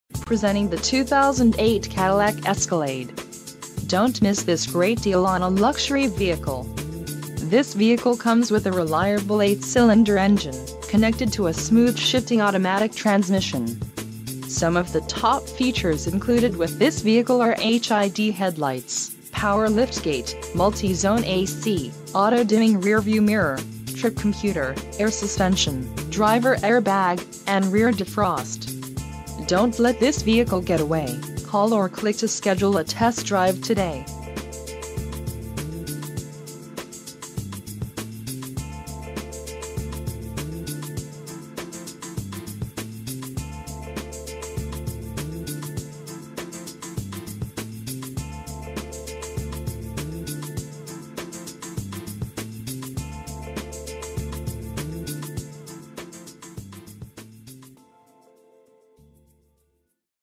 2009 Jeep Wrangler Unlimited Rubicon
4 Wheel Drive.. This trustworthy SUV seeks the right match*** This 2009 Jeep Wrangler Unlimited Rubicon won't last long at $2,735 below NADA Retail*** Includes a CARFAX buyback guarantee** SPECIAL ONLINE PRICING*** Stunning!!! Great safety equipment to protect you on the road: ABS, Traction control, Passenger Airbag, Front fog/driving lights, Stability control - With anti-roll control...It is nicely equipped: Power locks, Power windows, Convertible roof - Manual, Air conditioning, Cruise control... a Better buy from Benny Boyd